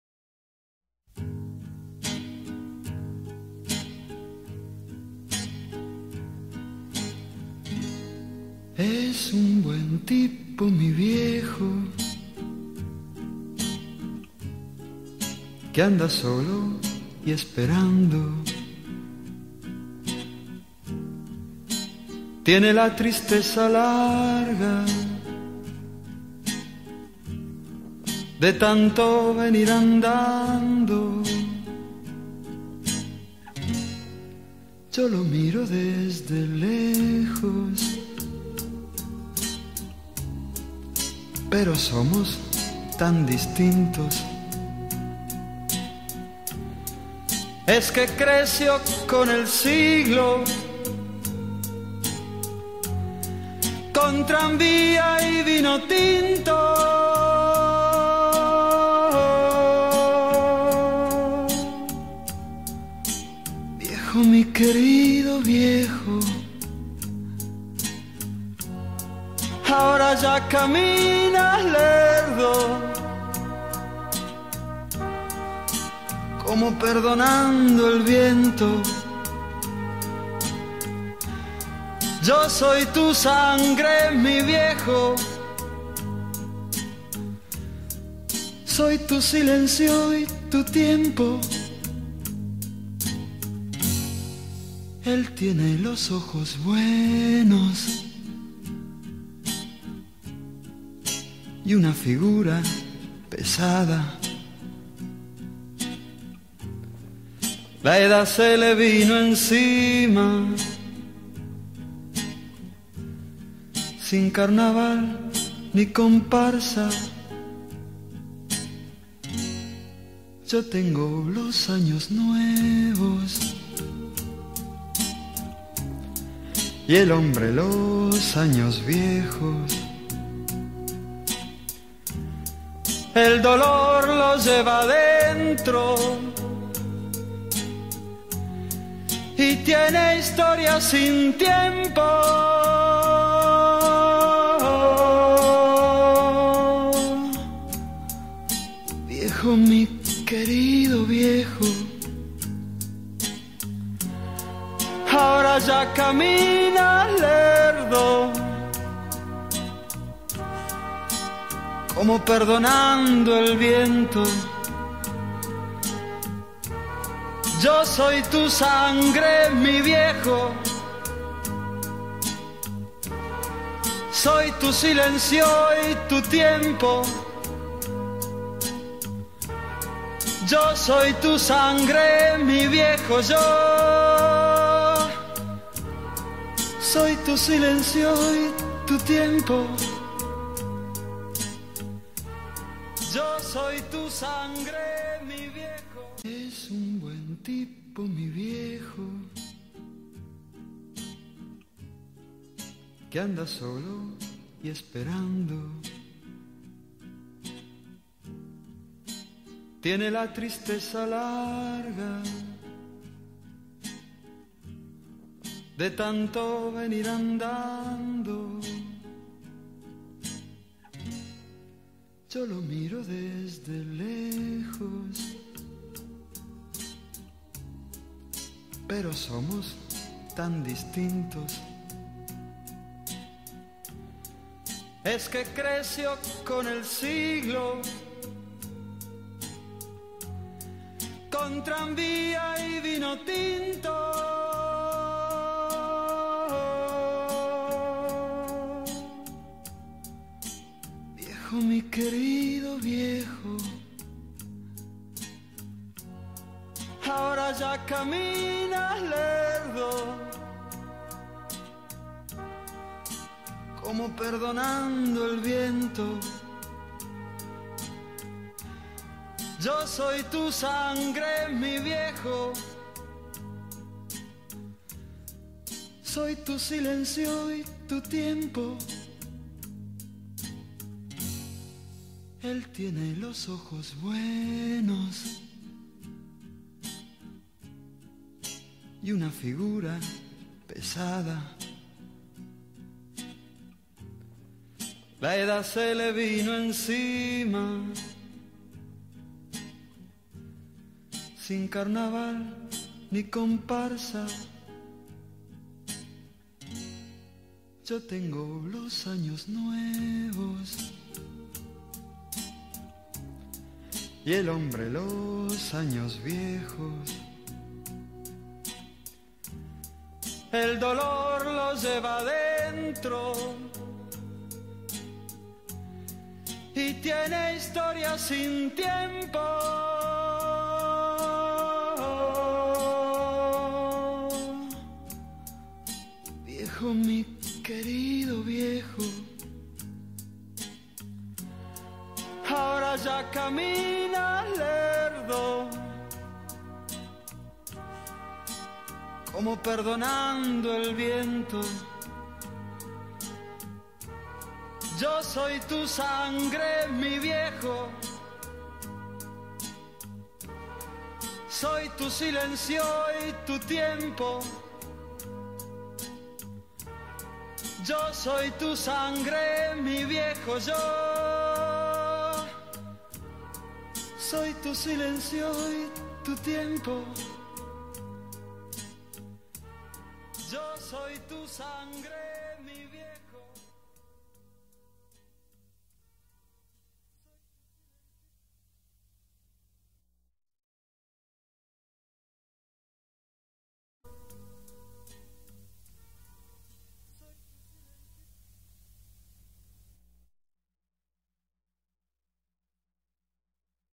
Mi padre
Este es mi padre gue tiene 5 meses fallesidos, ustedes no saben como lo extra�